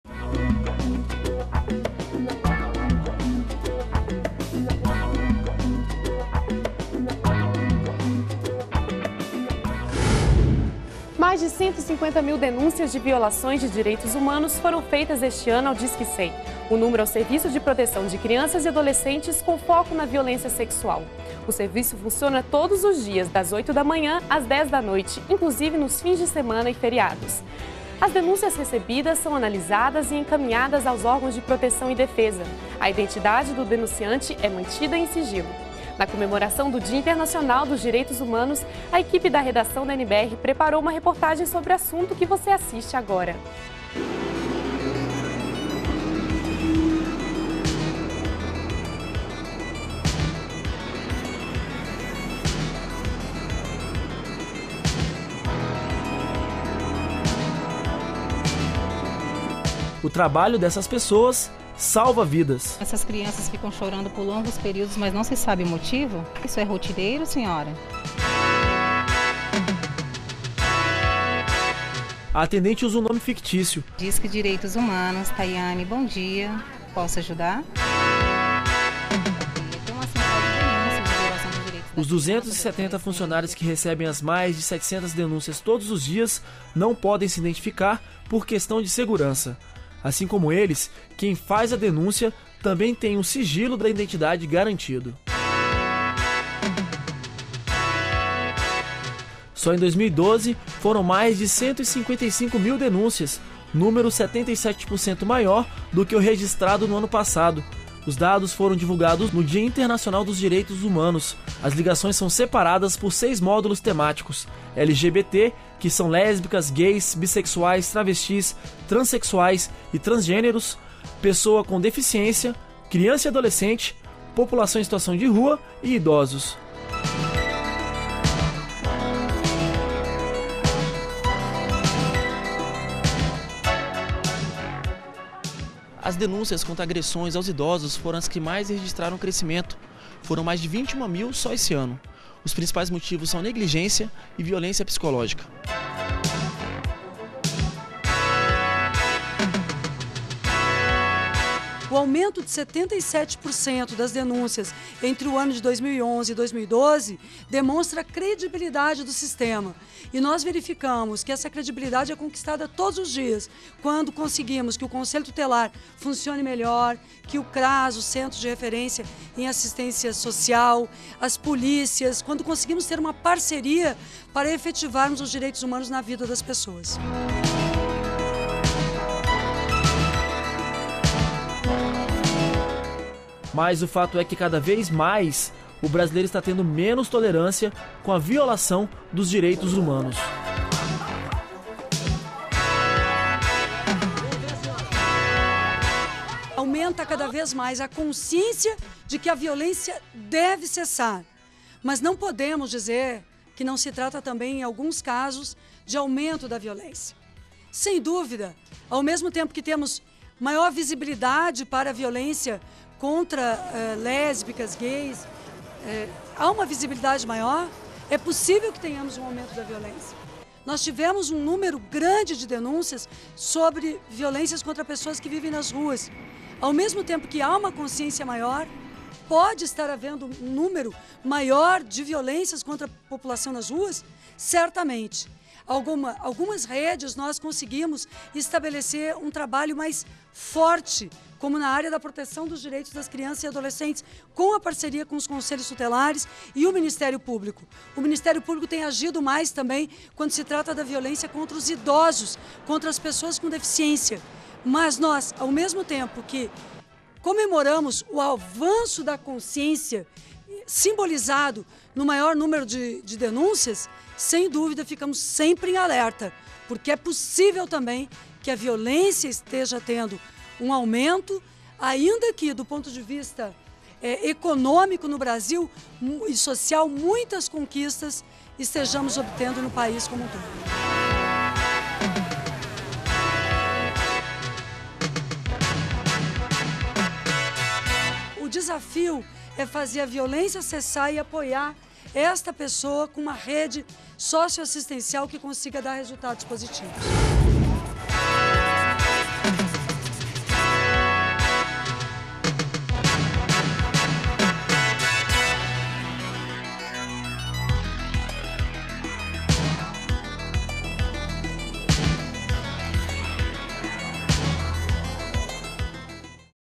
Disque 100 recebeu mais de 150 mil denúncias de violação dos direitos humanos em 2012
DA REDAÇÃO - 03.01.13: O serviço de proteção de crianças e adolescentes, o Disque 100, recebeu, em 2012, mais de 150 mil denúncias de violação dos direitos humanos. O serviço que funciona todos os dias das 8h as 22h e diariamente recebem denúncias que são analizadas e encaminhadas aos órgãos de proteção e defesa. Em comemoração ao Dia Internacional dos Direitos Humanos a equipe da Redação da NBR preparou uma reportagem sobre o serviço prestado pelo Disque 100.